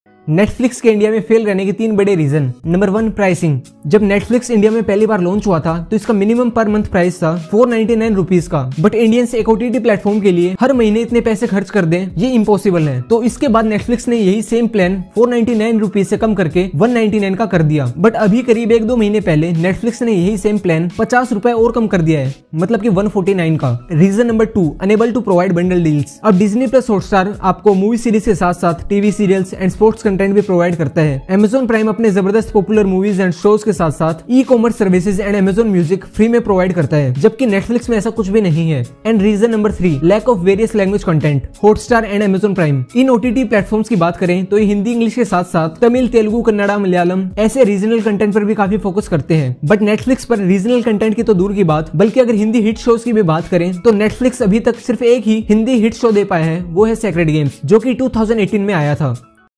Why Netflix Is Failing In India? Why Netflix Is Not Successful In India?
Why Netflix Is Failing In India? Why Netflix Is Not Successful In India? In this video I'm sharing 3 main reasons why Netflix Is Failing In India.